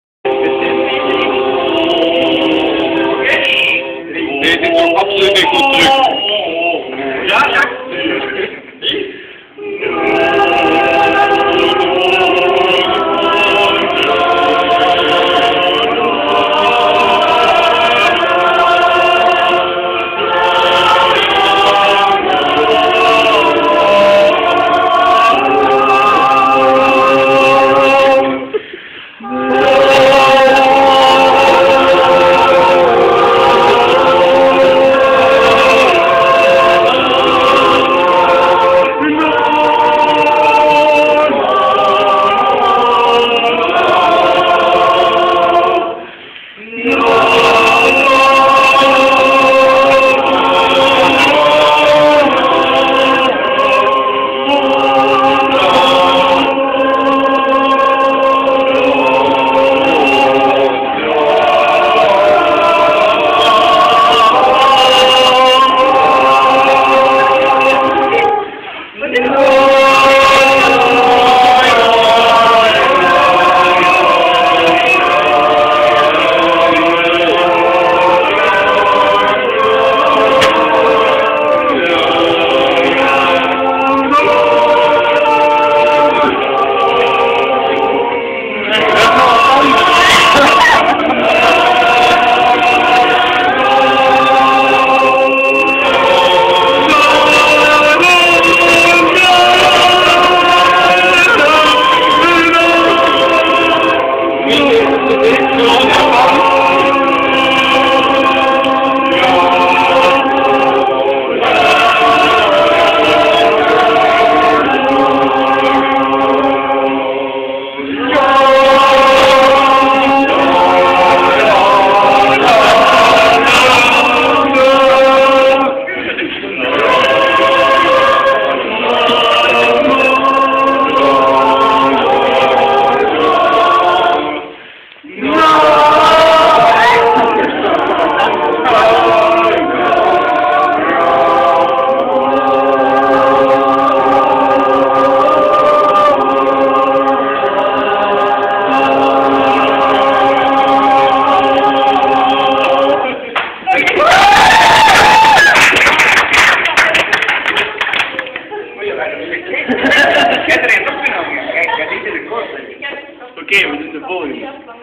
Koraalgezang door strijkersensemble Amadeus
We had a concert in a church on Suomenlinna, Helsinki. Underneath the church was a beautiful room and the contrabass player found a Finnish book with church songs in it. We couldn't help us trying to sing a song, or at least hum the notes. :)
I think we did a quite good job! xD